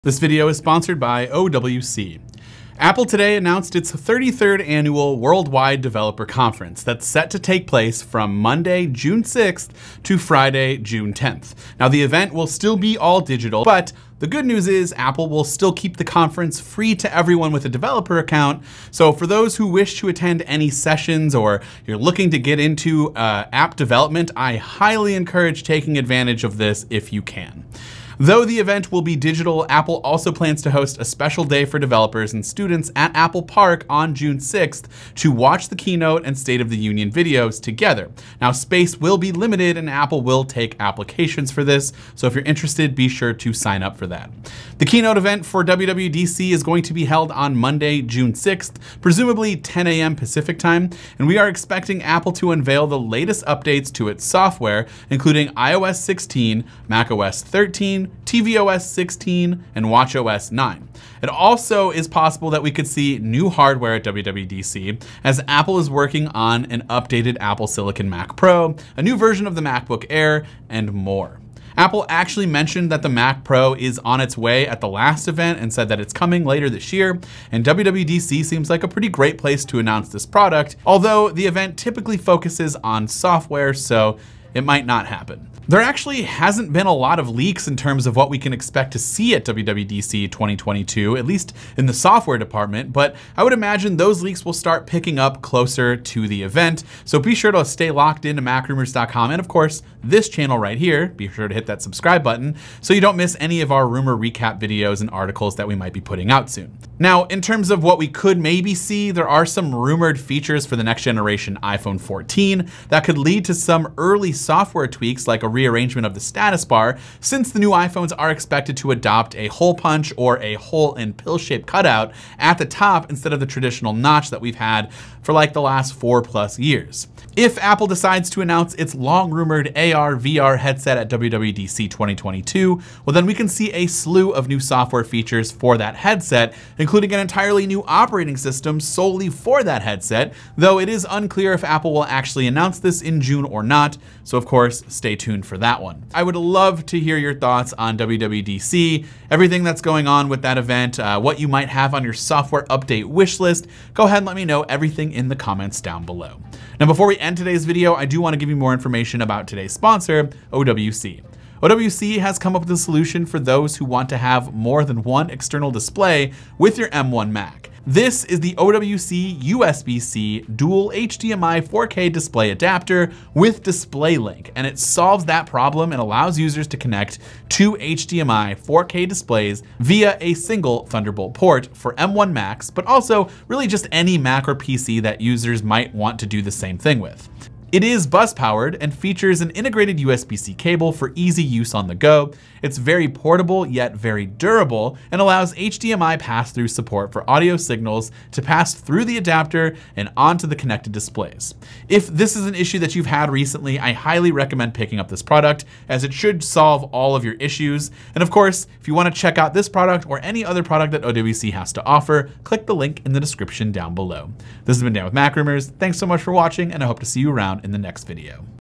Apple Announces WWDC 2022 (iOS 16, macOS 13, & More)
Apple today announced that its 33rd annual Worldwide Developers Conference is set to take place from Monday, June 6 to Friday, June 10. As with the last several WWDC events, the 2022 Worldwide Developers Conference will be held digitally with no in-person gathering.

Prior to 2020, Apple hosted WWDC at the McEnery Convention Center in San Jose, California, and thousands of developers and Apple employees attended, but in 2020, Apple introduced a free-for-everyone online event that will also be used in 2022.